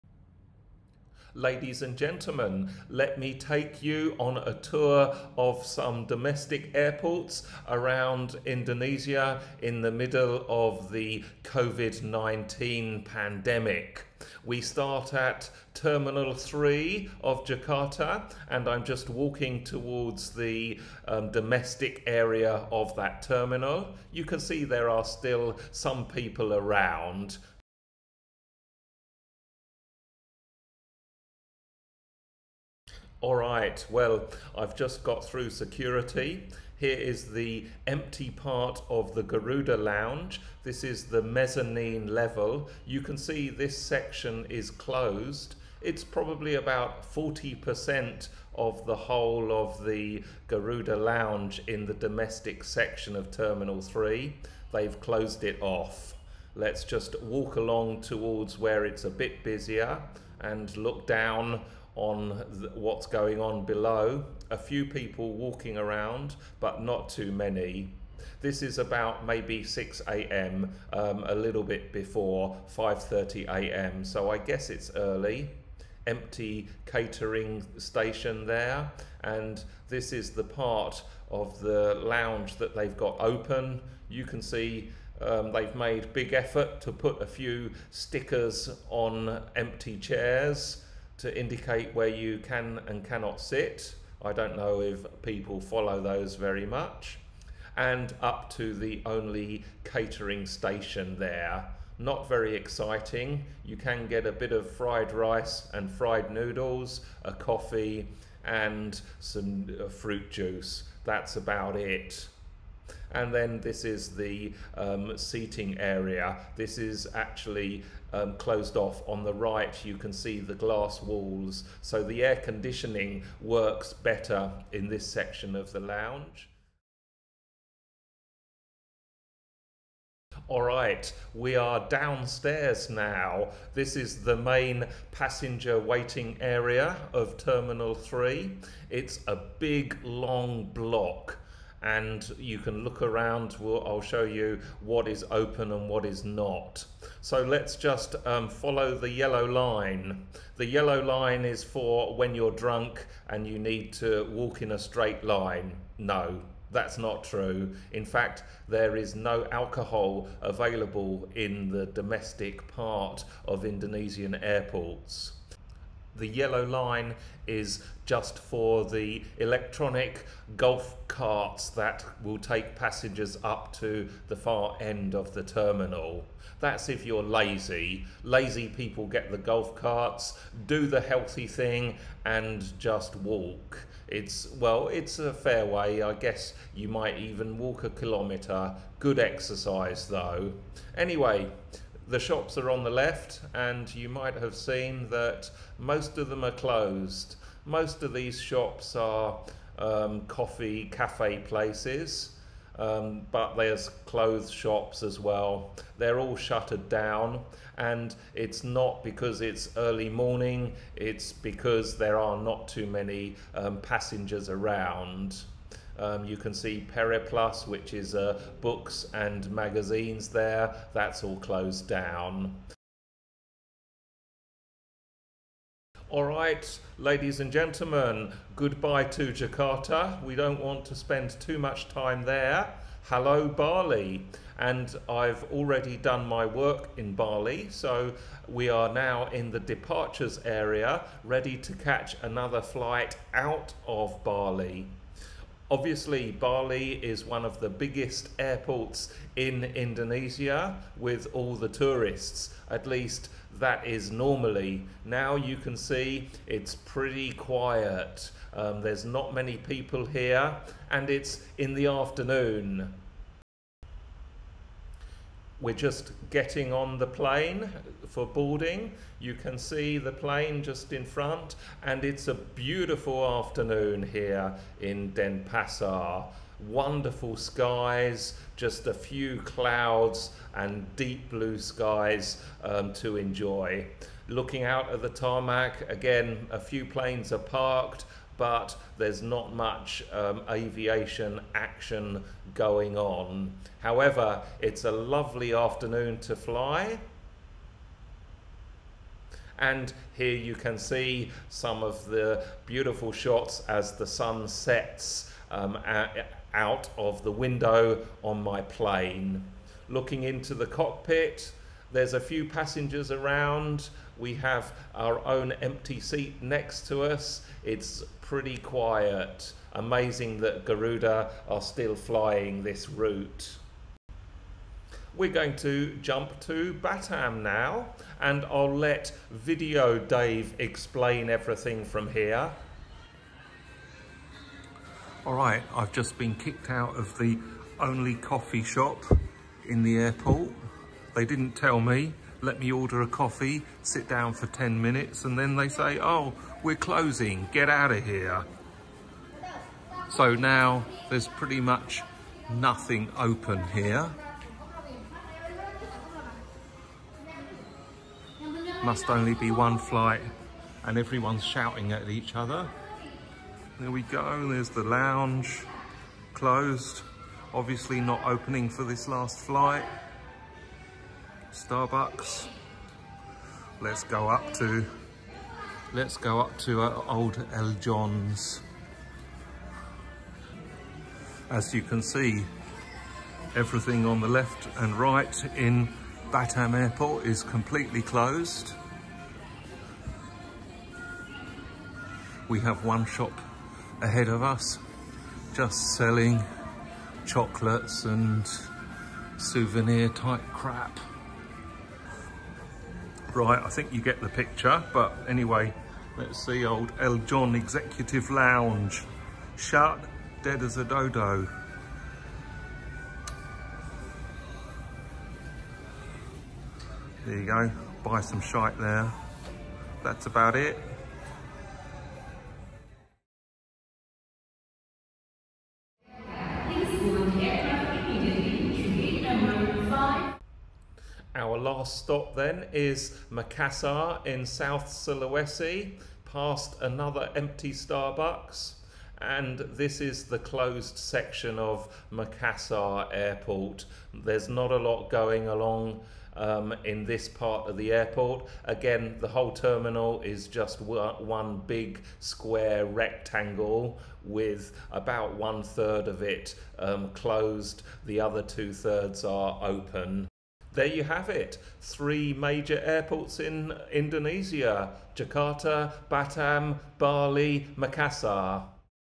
Airports in Indonesia: Late July & Early August 2020
A short look at how Indonesian aviation is handling air transport. We visit Jakarta, Bali, Batam & Makassar.